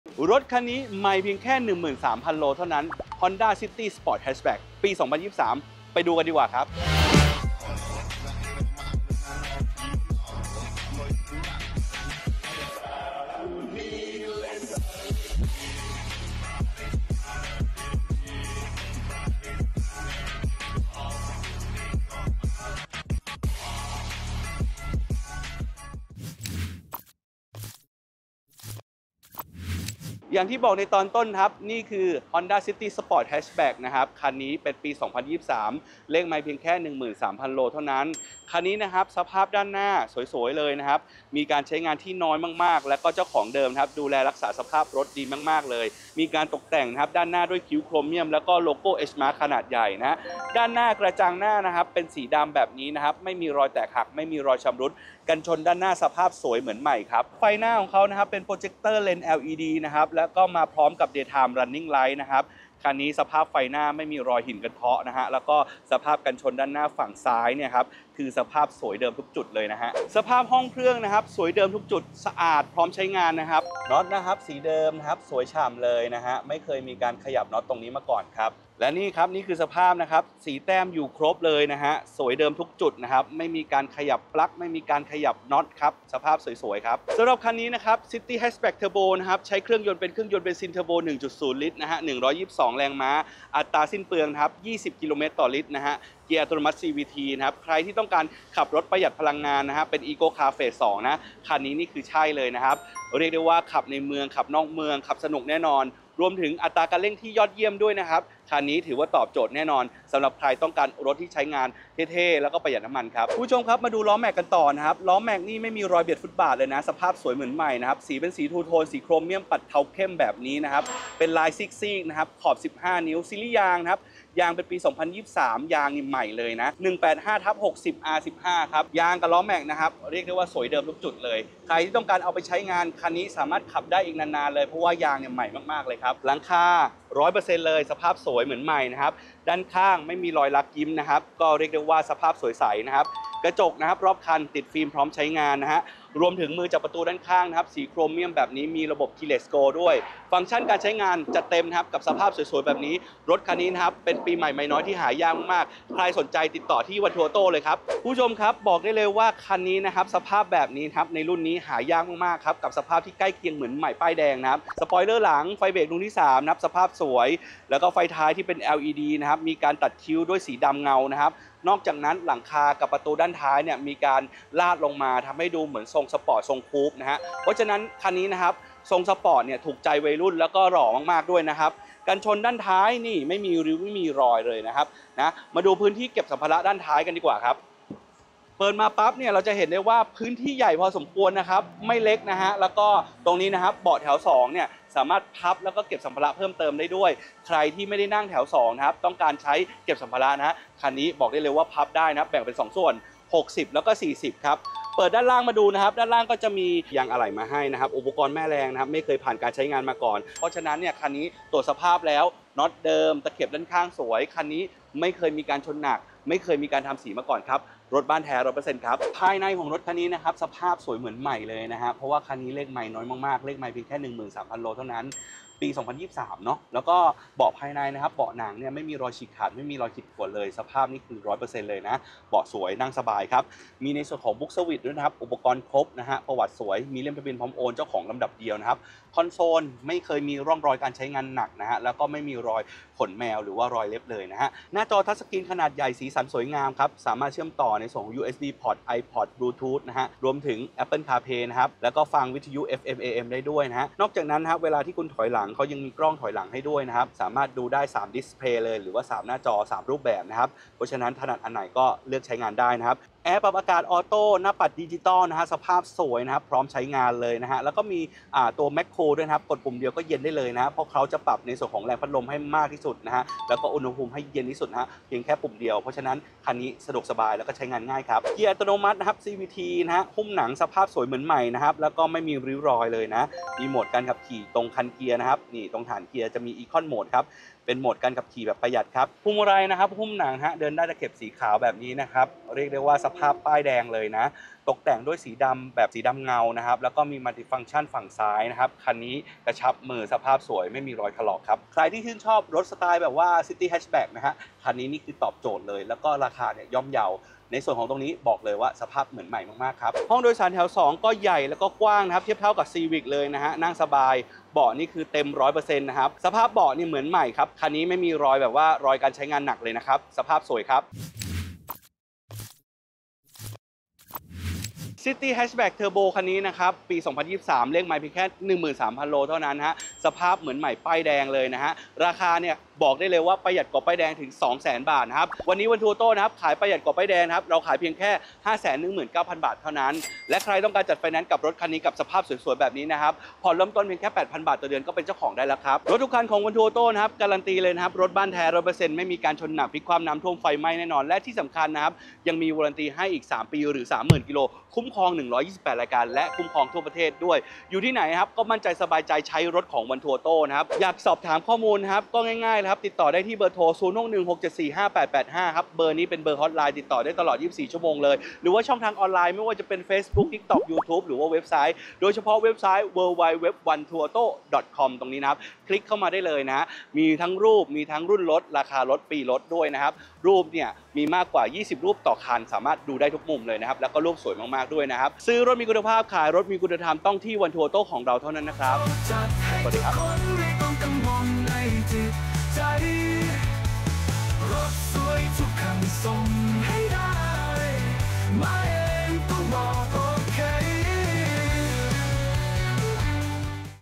[ONE2AUTO] รีวิว Honda City 1.0 Turbo SV Hatchback ปี2023 I EP. 424
Honda City 1.0 Turbo SV Hatchback ปี2023 เลขไมล์ 13,000 ก.ม.
ราคาป้ายแดง 679,000 บาท[ประหยัดได้ 160,000 บาท]
ราคา 519,000 บาท ผ่อน 8,000 บาท/เดือน

เครื่องยนต์เบนซินเทอร์โบ 1.0 ลิตร 122 แรงม้า
เกียร์อัตโนมัติ CVT
อัตราสิ้นเปลือง 20 ก.ม./ลิตร

Option✔️
⭐️ ไฟหน้า PROJECTOR LENS แบบฮาโลเจน
⭐️ ระบบปิดไฟหน้าอัตโนมัติ เมื่อดับเครื่องยนต์
⭐️ ไฟ DAYTIME RUNNING LIGHT แบบ LED
⭐️ ไฟท้าย แบบ LED
⭐️ ล้ออัลลอย ขนาด 15 นิ้ว
⭐️ สเกิร์ตรอบคัน และสปอยเลอร์หลัง
⭐️ ครอบกระจังหน้าดำเงา
⭐️ วัสดุตกแต่งภายในห้องโดยสาร สีดำเงา PIANO BLACK
⭐️ เบาะหนังแท้จากโรงงาน
⭐️ ระบบปรับอากาศแบบอัตโนมัติ
⭐️ กุญแจKeyless มือเปิดประตูโครเมียม
⭐️ ปุ่มPush Start
⭐️ กระจกมองข้าง ปรับและพับด้วยไฟฟ้า พร้อมไฟเลี้ยวในตัว
⭐️ พนักเท้าแขนตรงกลางเบาะนั่งคู่หน้า
⭐️ มาตรวัดเรืองแสงสีขาว พร้อมหน้าจอแสดงข้อมูลการขับขี่ MID
⭐️ หน้าจอเครื่องเสียงระบบสัมผัส ขนาด 8 นิ้ว รองรับระบบ APPLE CAR PLAY / ANDROID AUTO
⭐️ สวิตซ์ควบคุมเครื่องเสียง / รับสาย-โทรออก บนพวงมาลัย
⭐️ กล้องมองภาพขณะถอยจอด ปรับมุมมองได้ 3 ระดับ
⭐️ ช่องเชื่อมต่อ USB 2 ช่อง
⭐️ ช่องชาร์จไฟ 12V 1 ตำแหน่ง
⭐️ ที่บังแดด พร้อมกระจกแต่งหน้า ฝั่งคนขับ
⭐️ เสาอากาศแบบครีบฉลาม
⭐️ พร้อมสวิตซ์เปิดฝากระโปรงท้าย
ติดตามทุกความเคลื่อนไหว และสิ่งน่าสนใจใหม่ๆได้ที่
🚗one2auto
📍32/28 หมู่ 1 (ธนบัตรชัย 1) ถ.กาญจนาภิเษก ตรงข้ามสมาคมชาวปักษ์ใต้
⏰เวลาทำการ  09:00​​​ - 18:00​

🙋‍♀️ ปรึกษาเงื่อนไขการออกรถ
🙋‍♀️ เข้าชมรุ่นรถรายละเอียดเพิ่มเติม